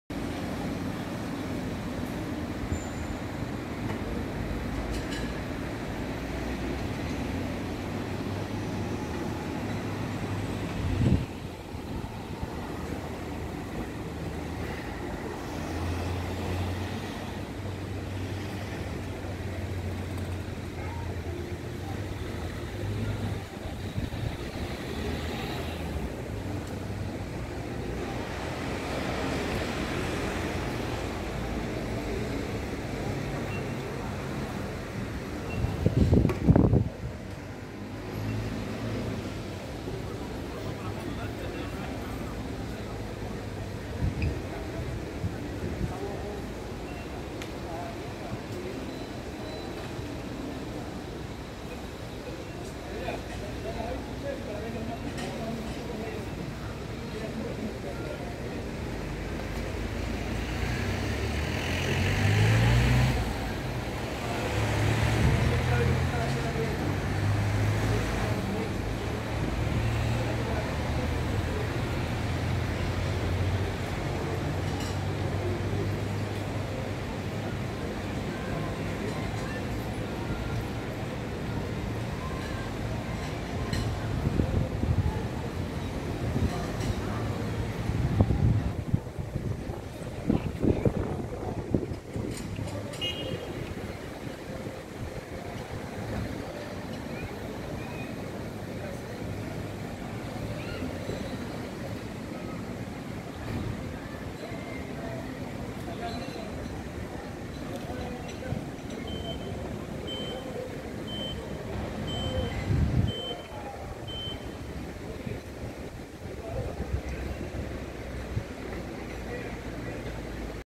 🔴 LIVE Puerto Rico Gran Canaria The Market March 2,2023
Live from Puerto Rico Gran Canaria Shopping Centers Today